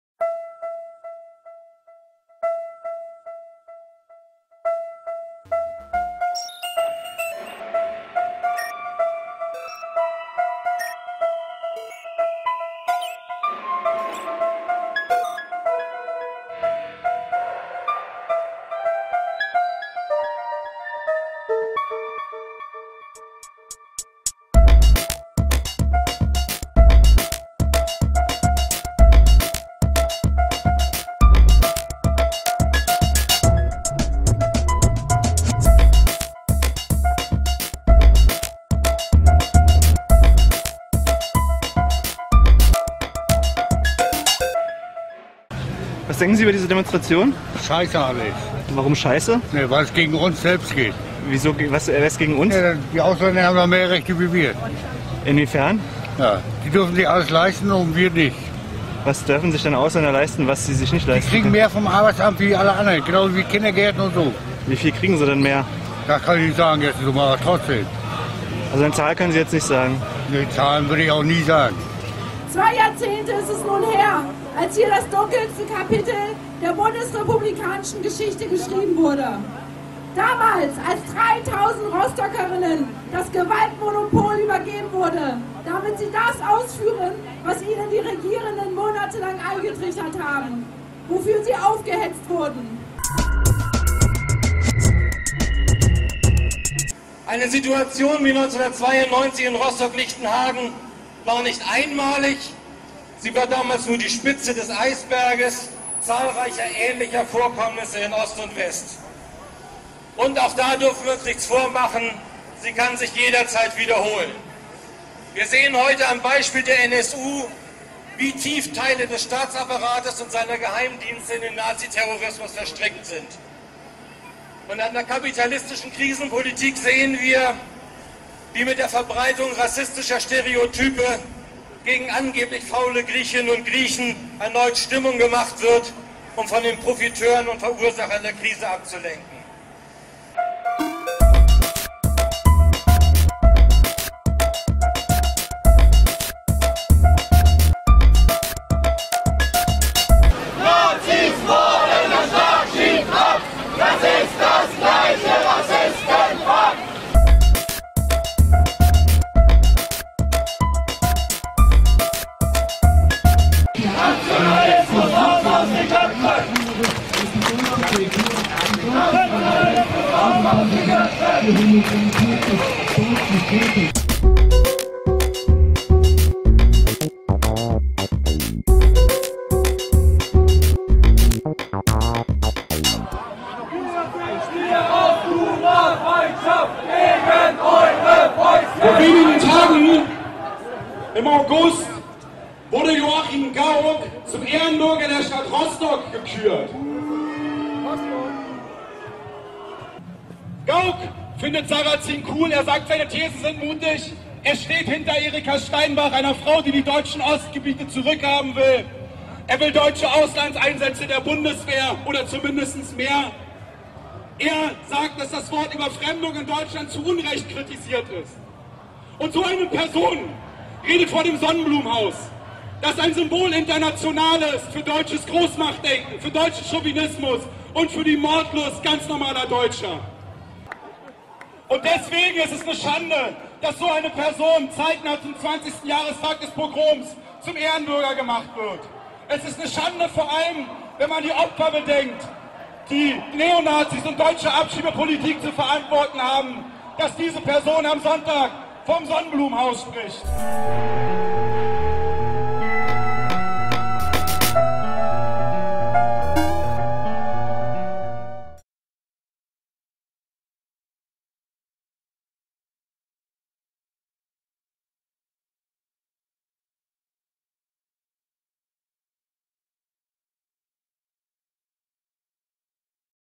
Filmpiraten - 20 Jahre nach dem Pogrom von Rostock-Lichtenhagen
Unter dem Motto „Das Problem heißt Rassismus" demonstrierten am 25. August 2012 ca 5000 Menschen in Rostock-Lichtenhagen. 20 Jahre ist es nun her, das ein Mob aus Nazis und Rassisten fünf Tage lang die Zentrale Aufnahmestelle für Asylbewerber_innen(ZAST) belagerten und diese mit Steinen angriffen und mit Molotococktails in Brand setzten. Nur durch ein Wunder gab es damals keine Toten. Die Demonstration gedachte dem 20. Jahrestag, aber richtete sich auch gegen das offizielle Gedenken der Stadt Rostock mit Bundespräsident Joachim Gauck.